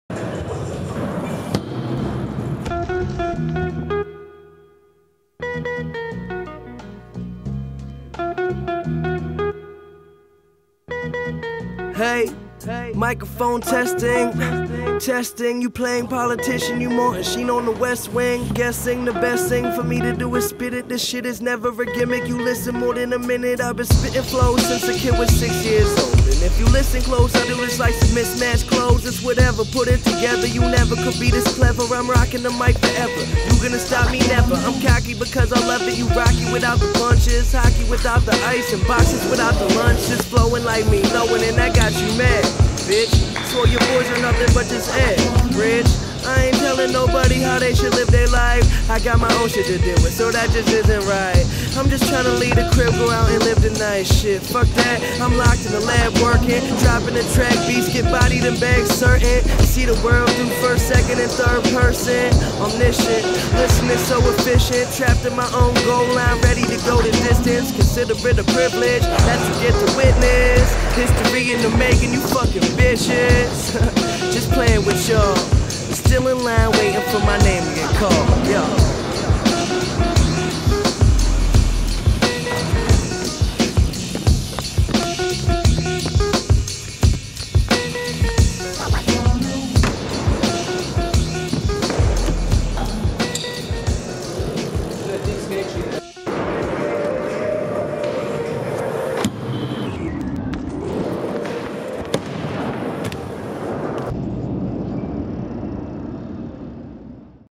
PARBMX / AIVARS JUSTOVICS @ THE SPOT CENTER
Aivars teamed up with his long time friend Renars Zvirgzdins in order to catch a few after work combos at his local skatepark in Riga, Latvia "The Spot". The end result is this fun piece of trickery filmed during evening hours of January, 2019.